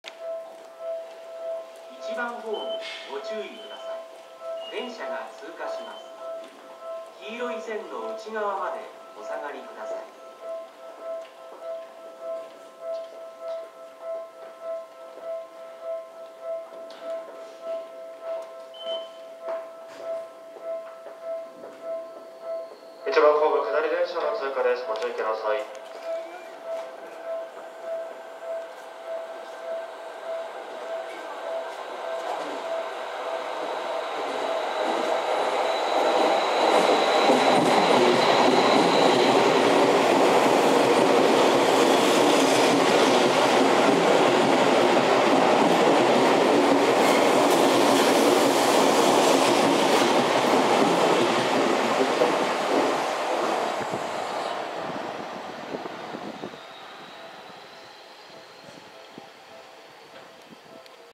西武30000系38116編成快速急行本川越行き＠新井薬師前駅
東京都中野区 西武新宿線新井薬師前駅にて